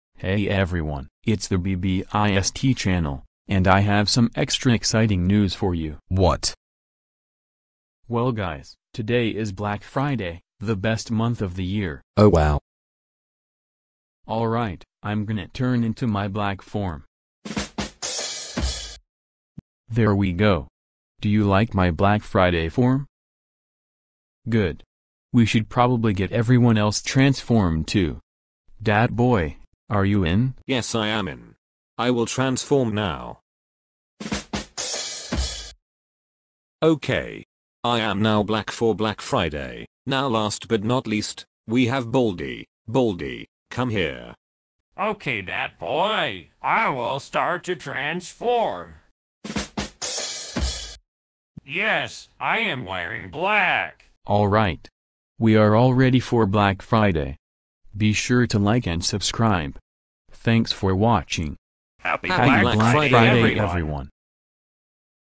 Happy Black Friday 🖤
Happy Black Friday Month Everyone! let's see baldi, dat boi, and me dressing up for the month! enjoy the video!